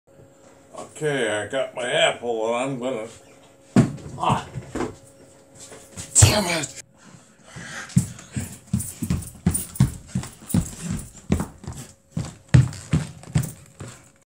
apple
ok I dropped an apple and it is a big deal